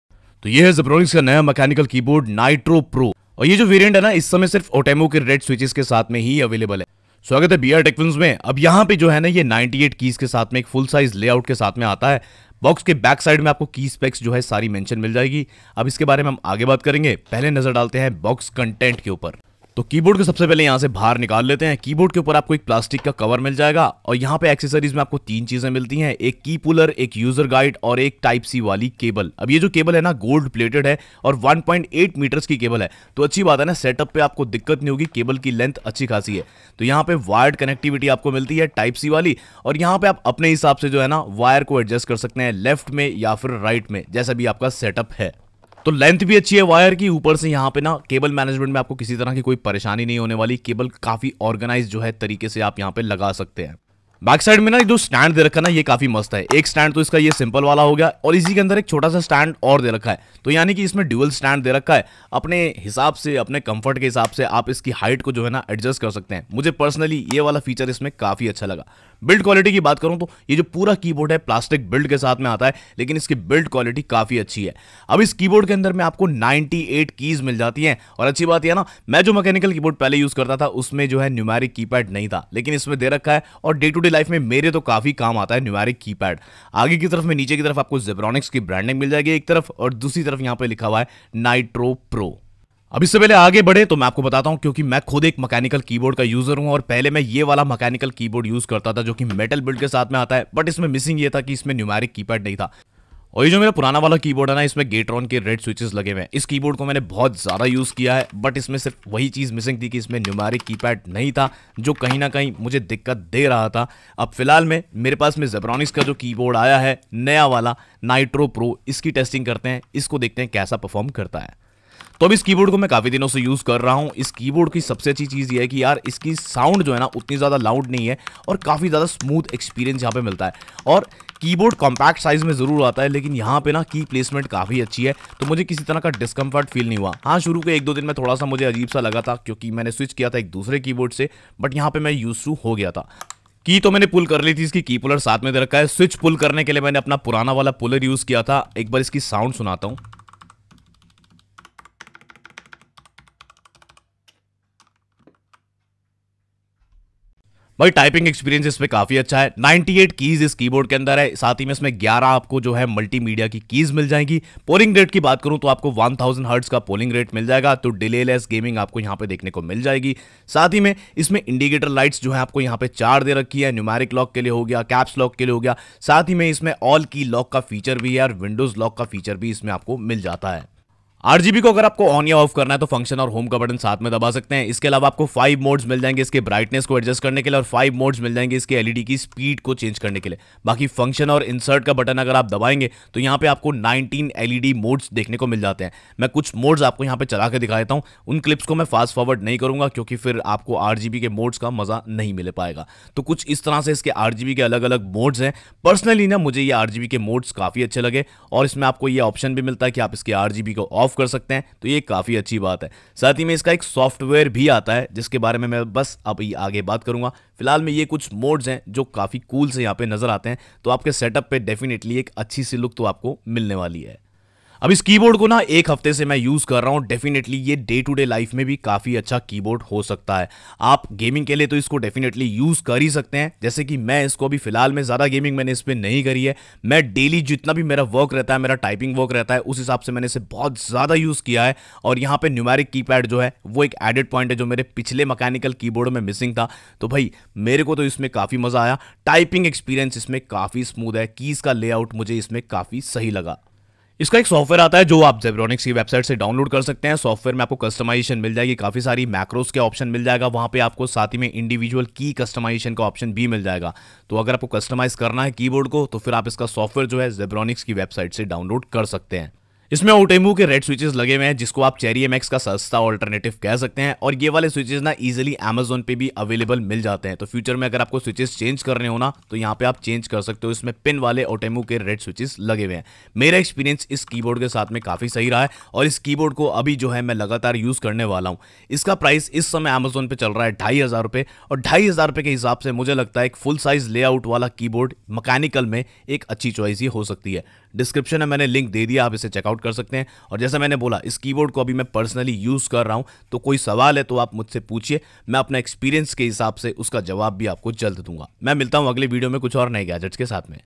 Zebronics Nitro PRO Outemu RED Switch Gaming Mechanical Keyboard | BR Tech Films
My Review of ZEBRONICS Nitro PRO Outemu RED Switch Gaming Mechanical Keyboard with Software Customization for Macros, RGB LED, 1000Hz Polling Rate, Detachable Braided Type C Cable & Key Removal Tool
Best Buy

1.8 meters Gold-plated USB - Type C detachable braided cable for connection
Suitable for Gaming, Typing works, Office and everyday usage.
Outemu RED switches Less noisy mechanical switches and smooth typing experience without fatigue. Comfortable and Tactile.
Customizable Macros and 19 LED modes
1000Hz polling rate for delay less performance - 50 Million key lifespan.
98 keys Full size keyboard layout, 11 integrated multi-media keys, All key lock function.
this is the best gaming mechanical keyboard under RS 2500

Note : I have expressed my personal experience on the product as a user all opinion are mine

This video is created by BR Tech Films